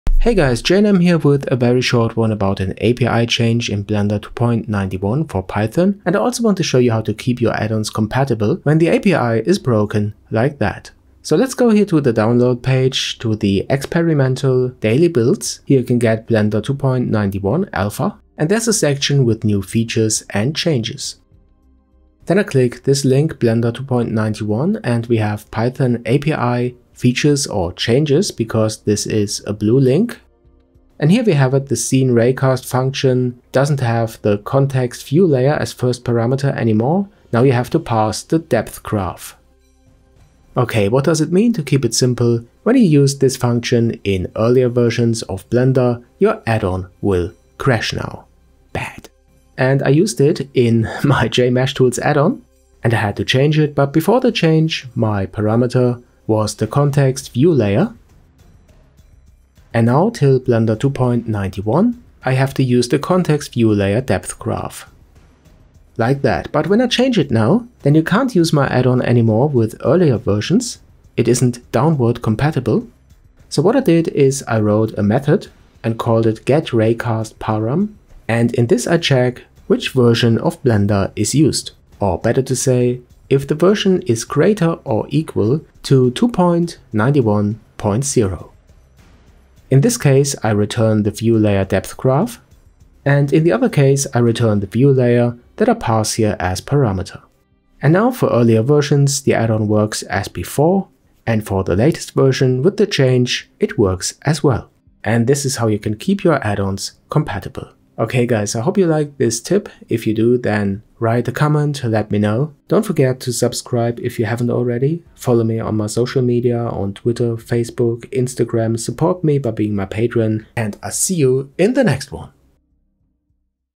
Blender 2.91 Python API Change : How to keep addons compatible
The latest experimental Blender 2.91.0 version has a breaking API Change for the scene.raycast function. In this video I can show you how to change the signature from the view_layer to the depsgraph - but I also also want to show you how to keep you addons downwards compatible.